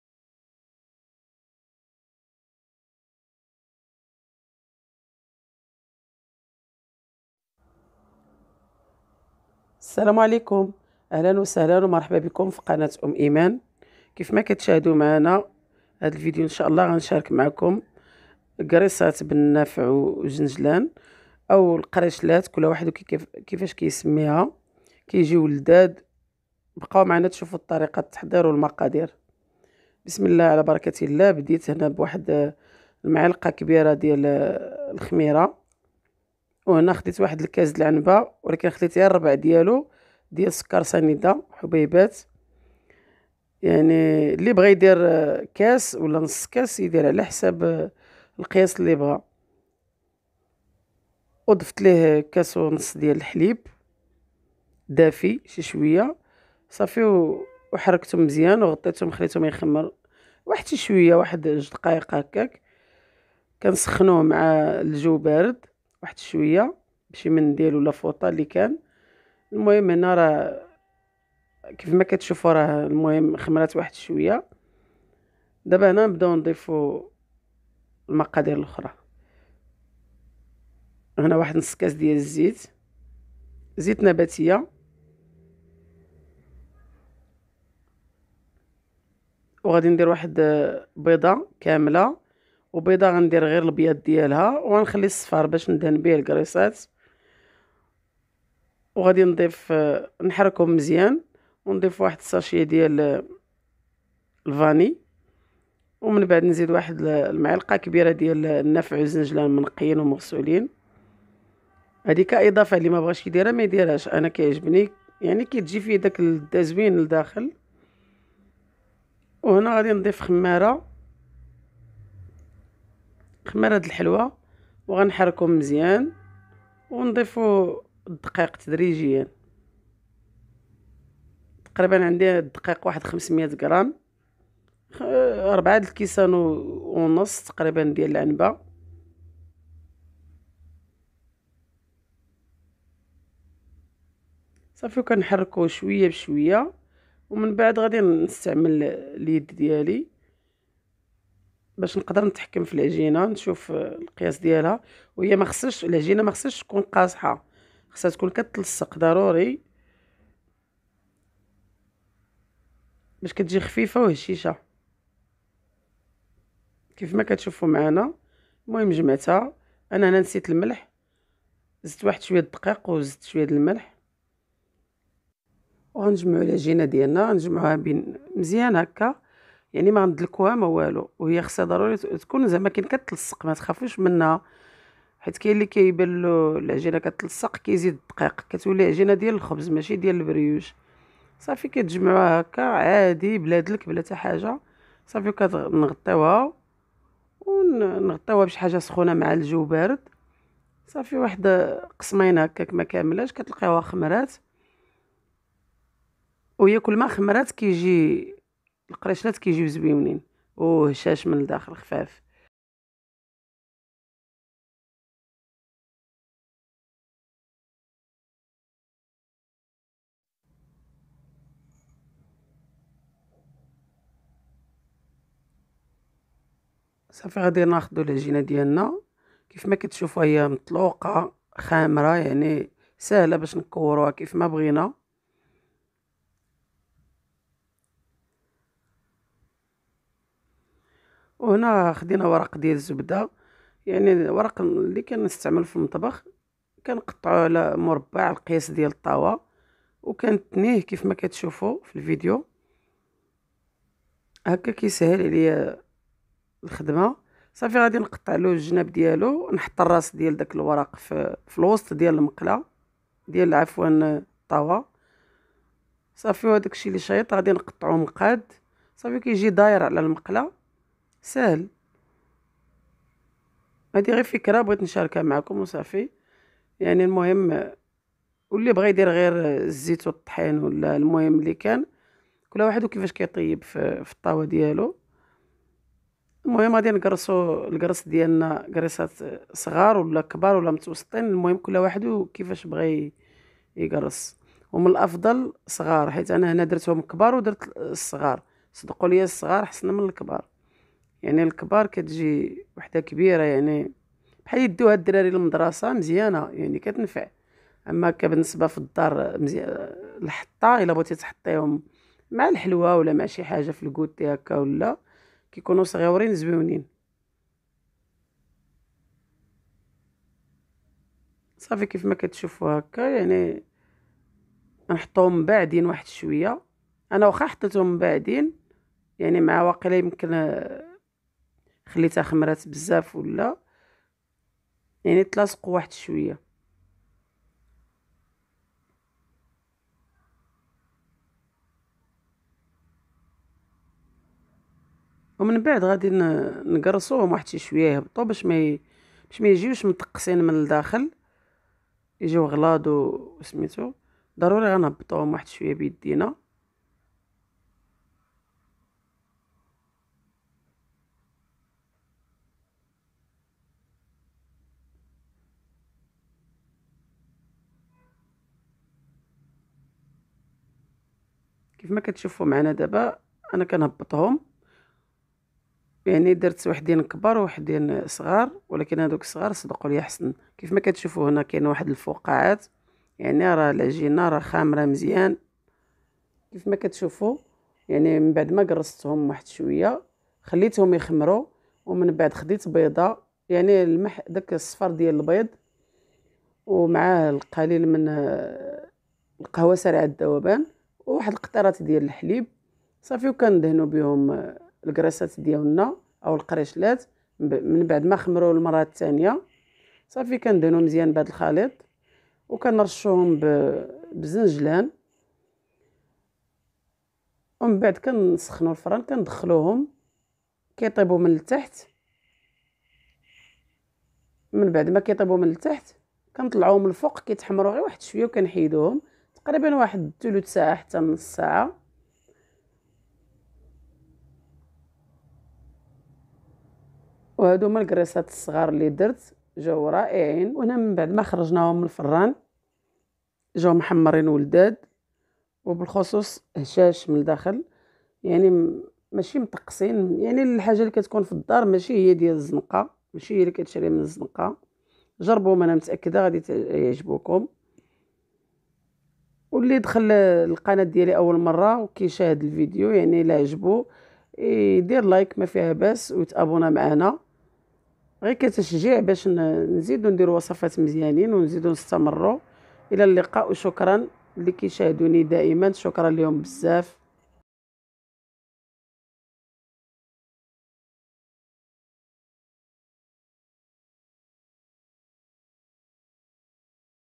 كرص/قريشلات/بريوش بالحليب رطب و بمقادير مضبوطة و بدون دلك و لا مجهود | ناجح 100%
اهلا بكم في قناتي
اليوم فيديو جديد اتمنى ان يعجبكم .
شكرا على تتبعكم و تعليقاتكم المحفزة

كرص بالحليب
بريوش سهل
بريوش اقتصادي
الكرص بالسمسم
الكرص بالنافع